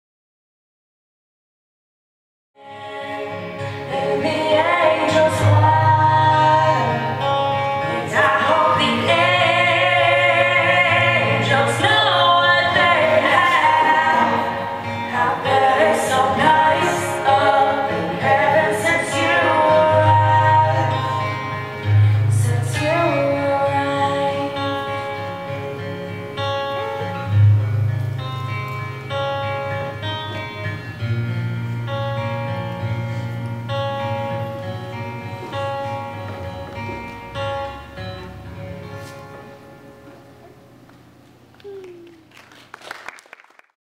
Memorial
My daughters were part of a group that did Polynesian Dance as Worship. The High School girls decided to do a Memorial dance as a way to bond as it was something they all had in common. losing a loved one. They gathered pictures from the audience of fellow lost loved ones, this was the ending of that dance.